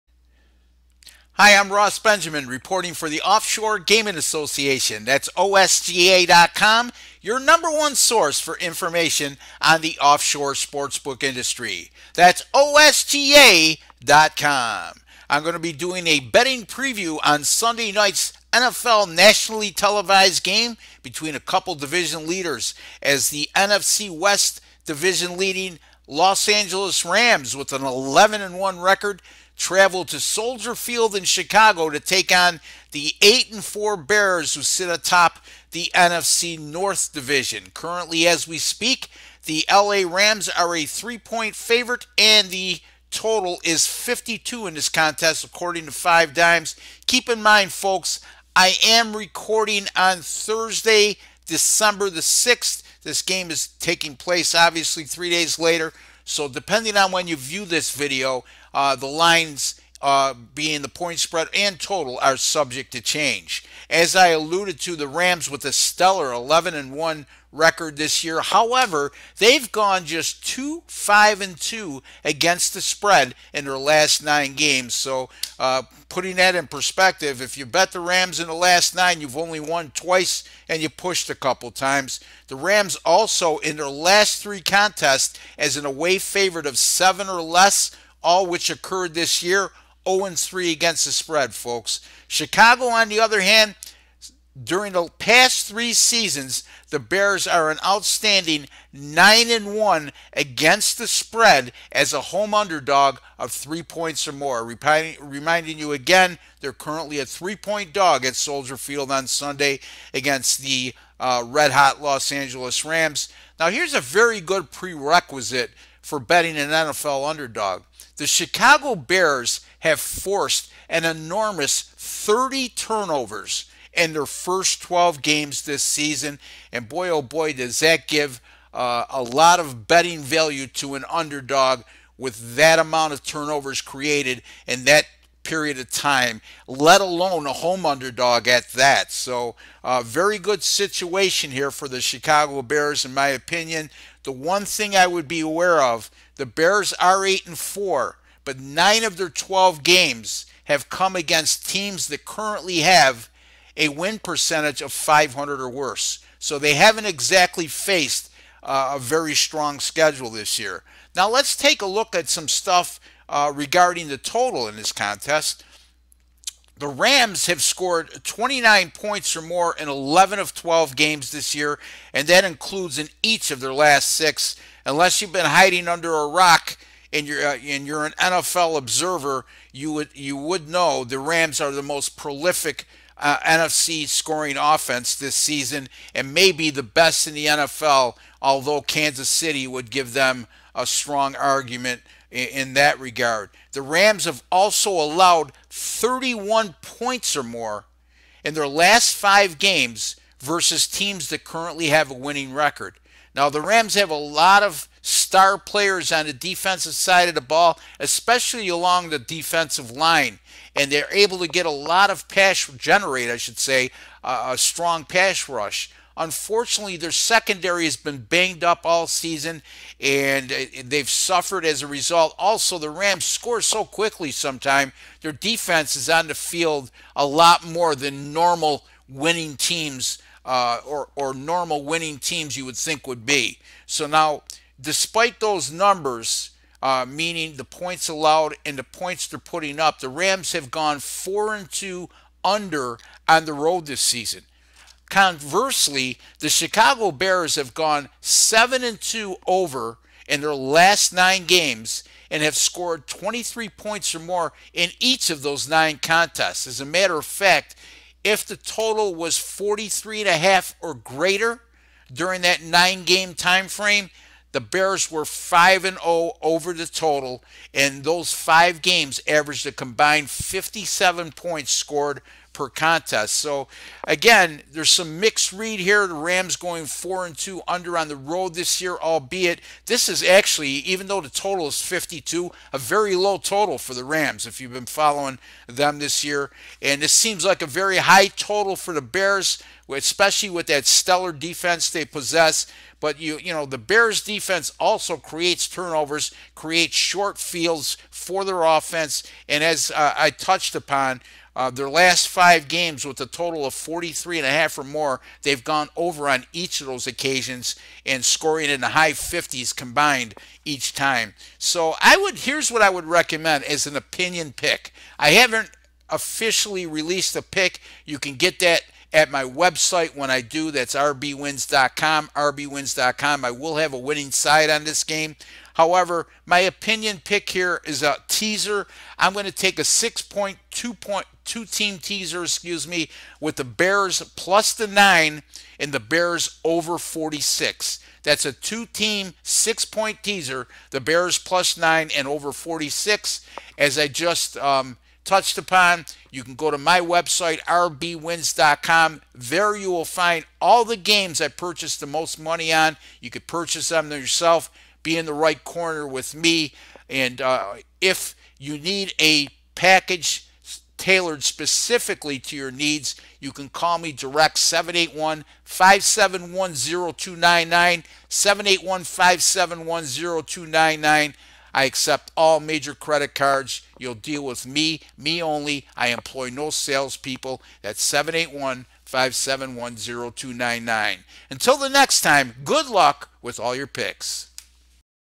NFL Betting Preview: Rams vs. Bears
Prominent professional sports provides a betting preview on Sunday night's NFL nationally televised game between the Rams and Bears. Ross concludes the video with a suggested 2-team 6-point teaser for this contest.